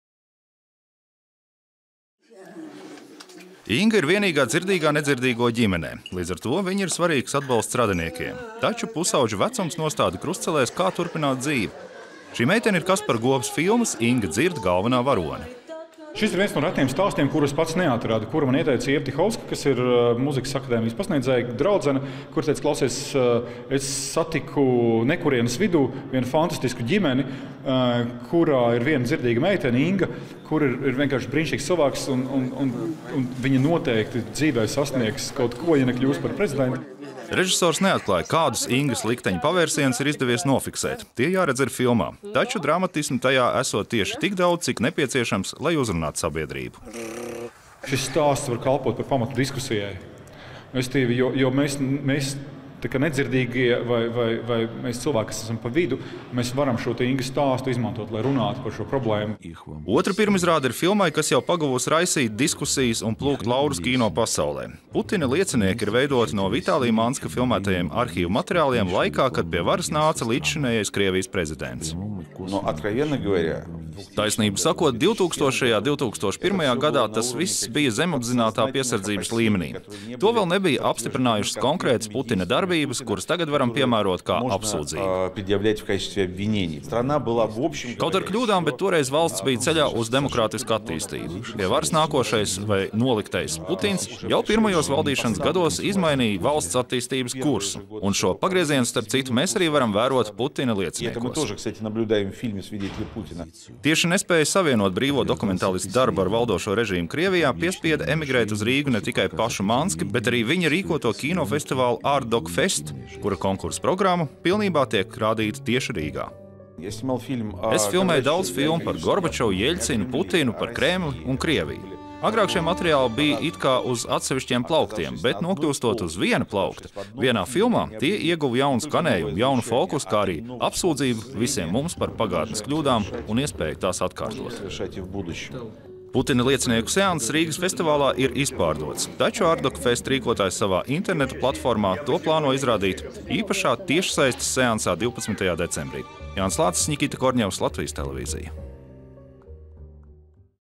Šodien Riga IFF programmā – divas pirmizrādes
Rīgā turpinās Starptautiskais kino festivāls. Šovakar tā programmā divu filmu pirmizrādes, kas piesaistīja arī kultūras ziņu uzmanību. Viena no tām ir Vitālija Manska dokumentālā filma “Putina liecinieki”, kas iekļauta Pasaules dokumentālā kino programmā, bet otra – Kaspara Gobas “Inga dzird”, ko demonstrēs Baltijas kino programmā. Ar abu filmu režisoriem tikās Jānis Lācis